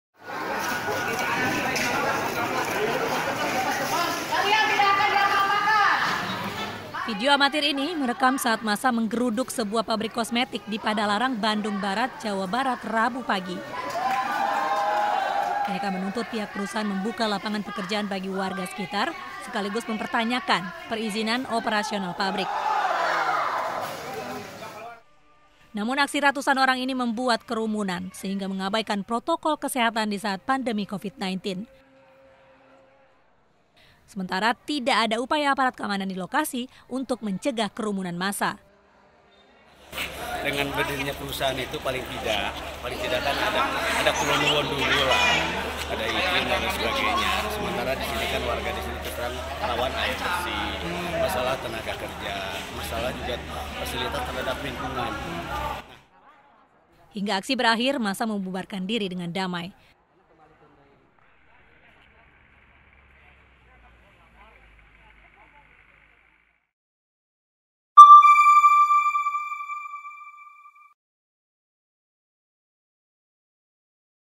Ratusan Warga Geruduk Pabrik Kosmetik di Padalarang Tuntut Pekerjaan
Ratusan orang menggeruduk sebuah pabrik kosmetik di Padalarang, Jawa Barat, untuk menuntut pekerjaan.

Aksi ini membuat kerumunan  sehingga mengabaikan protokol kesehatan di masa pandemi Covid-19.

Android: inews.id/android
iPhone: inews.id/ios